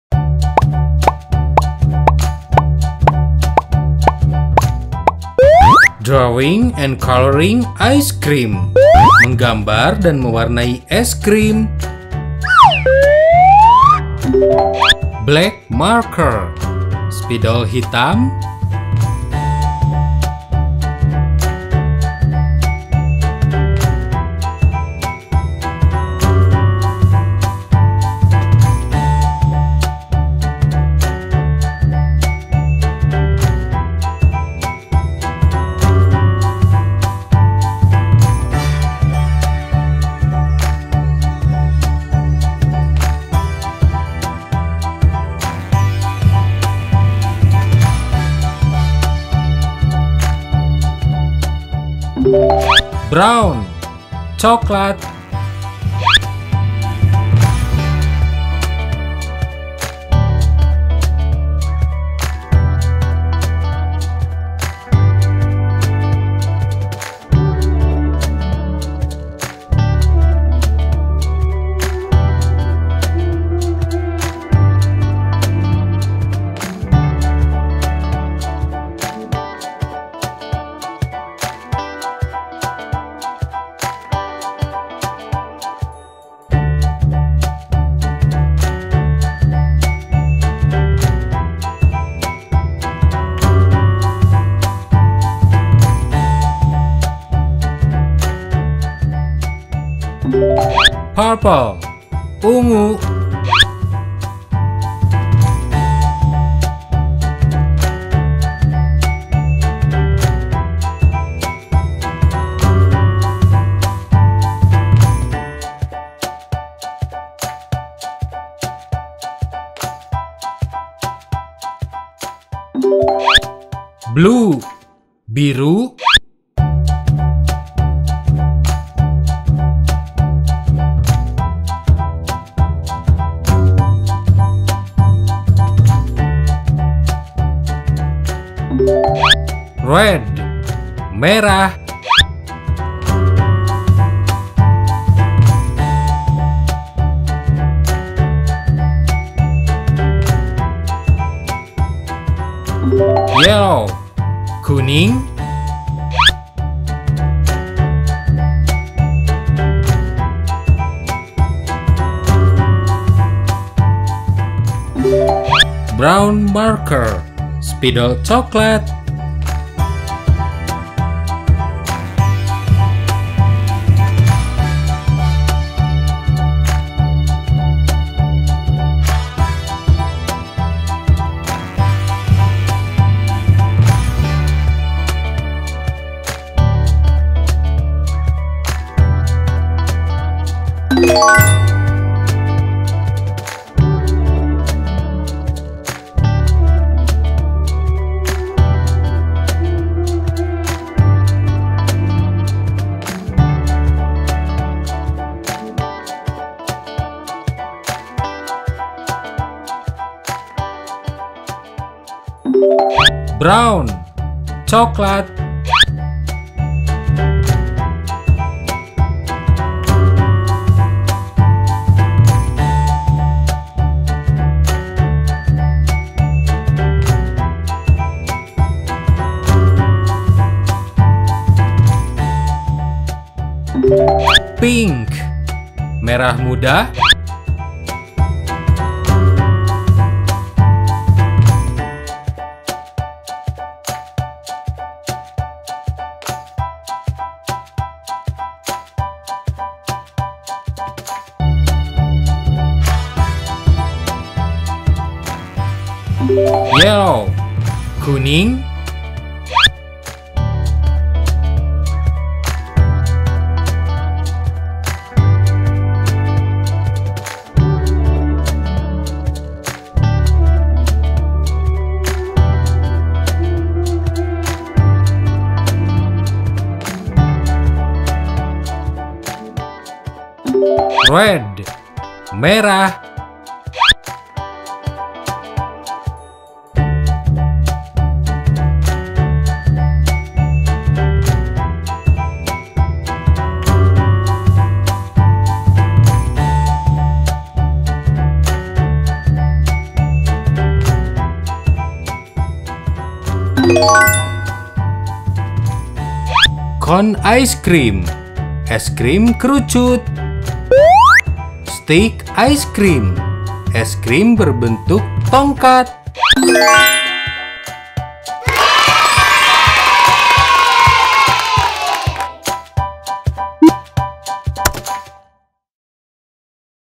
Menggambar - Melukis dan Mewarnai Es Krim Stik dan Es Krim Kerucut | Belajar Warna Bahasa Inggris
Video ini yang berjudul Menggambar dan Mewarnai Es Krim Stik dan Es Krim Kerucut | Belajar Warna Bahasa Inggris Untuk Anak, bertujuan agar anak-anak bisa menggambar dan mewarnai es krim kerucut dan es krim stik sambil belajar mengenal warna-warna dalam bahasa inggis. Selamat menyaksikan.